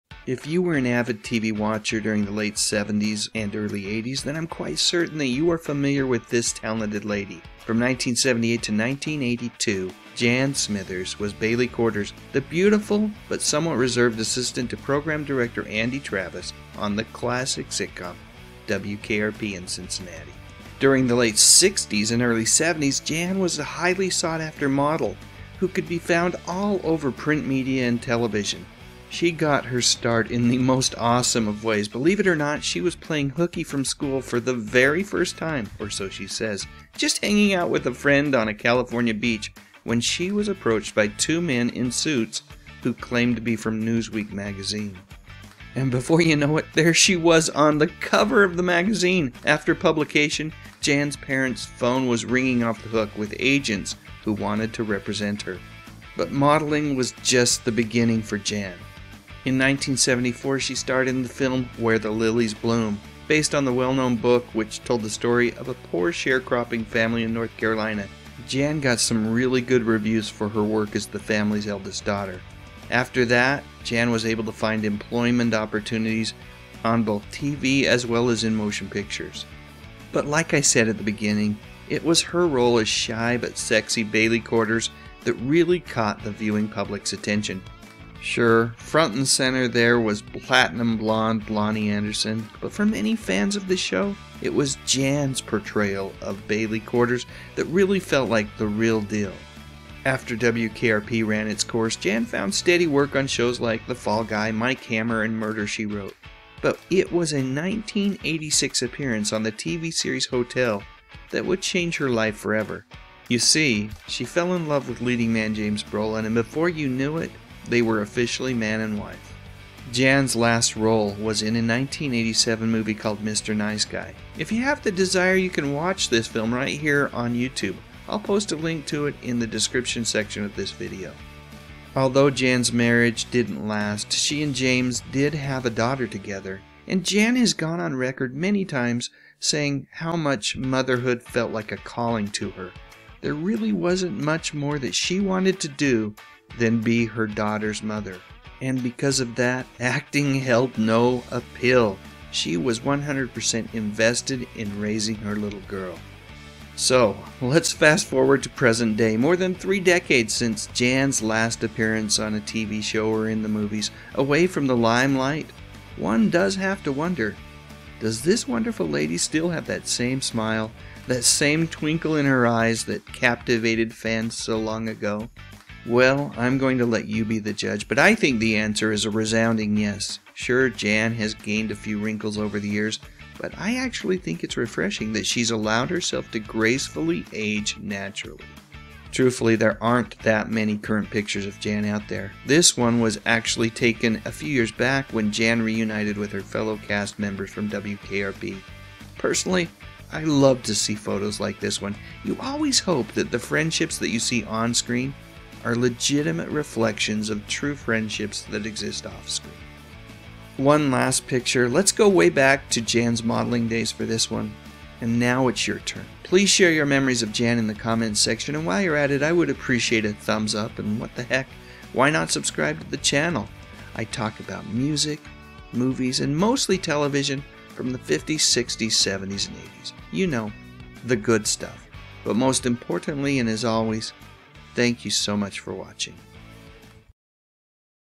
Here's What JAN SMITHERS Looks Like Now - WKRP's Bailey Quarters!
If you watched TV during the late 70s and early 80s, you knew who Jan Smithers was...especially if you were a fan of the classic TV show, "WKRP in Cincinnati" where she played the beautiful but somewhat reserved Bailey Quarters.  Here are a few of my memories of Jan's career plus a brief update.